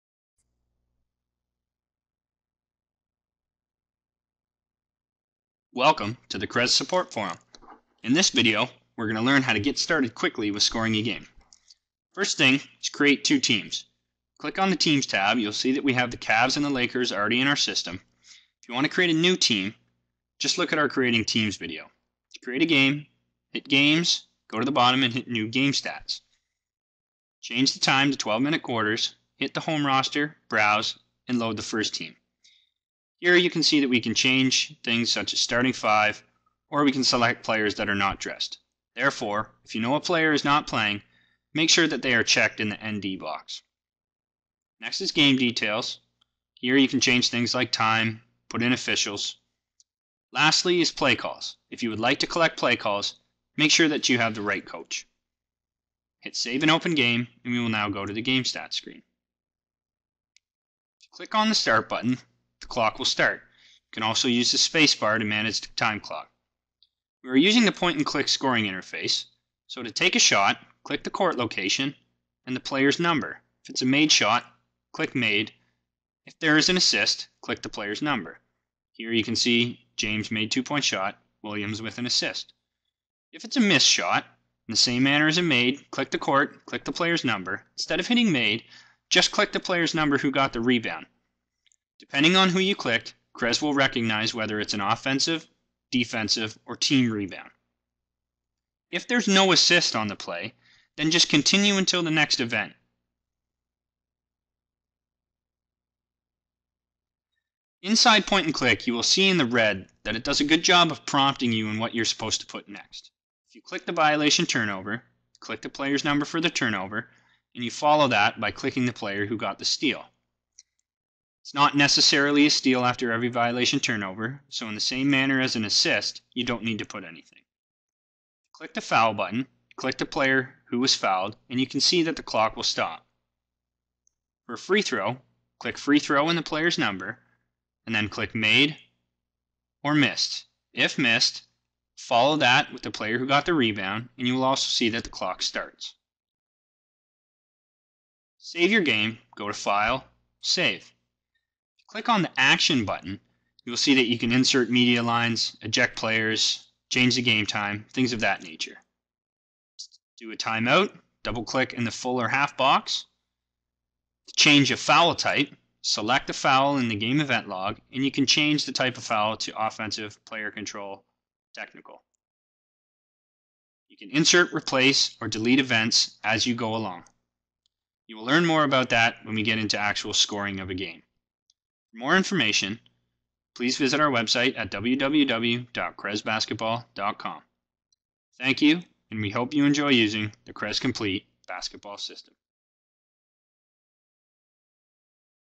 CREZ Basketball - Statistics Made Easy
CREZscorer allows coaches to easily generate and analyze full-game statistics. Get set up quickly and minimize frustration with the FREE CREZscorer package.